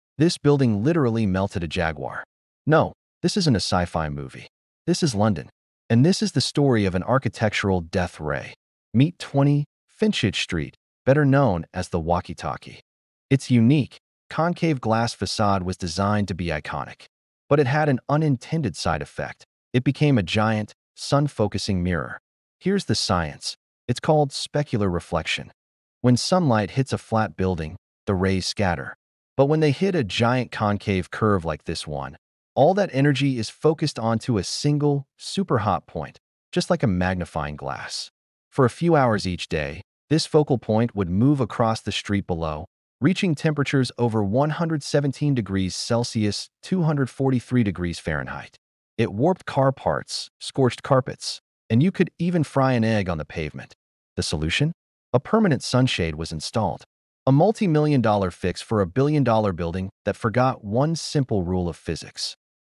This building literally melted a Jaguar
In London, a skyscraper accidentally turned sunlight into a death ray.
The curved glass of 20 Fenchurch Street focused sunlight so intensely it melted parts of a parked Jaguar car.
The building wasn’t broken — physics did exactly what it’s supposed to do.